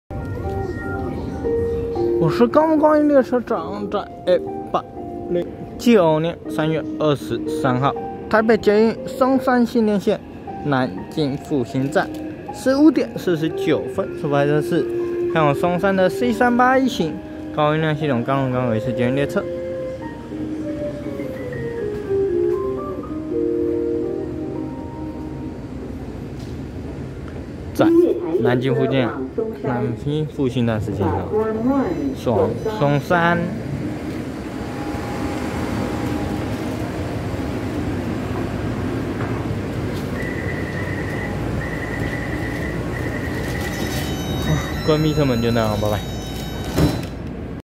台北捷運松山新店線南京復興站15:49分拍攝開往松山的C381型高運量系統鋼輪鋼軌式捷運列車
109/3/23台北捷運松山新店線南京復興站15:49分拍攝開往松山的C381型高運量系統鋼輪鋼軌式捷運列車